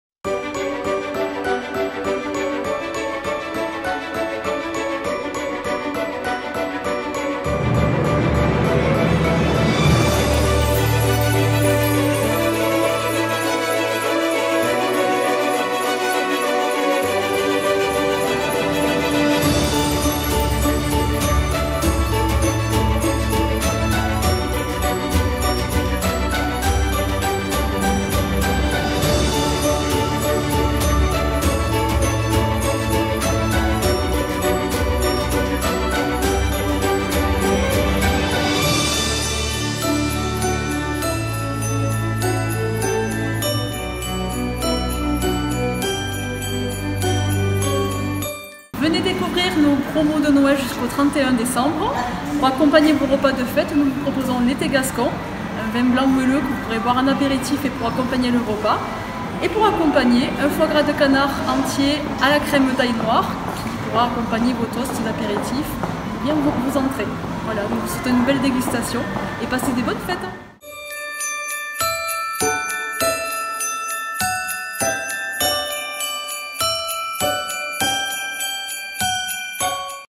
La magie de Noël est arrivée dans votre Boutique Gourmande Auch
Stéphanie et son équipe sont à votre disposition tous les jours, pour vous aider à confectionner des cadeaux gourmands.🎁🎊
Le masque a été enlevé uniquement lors du tournage .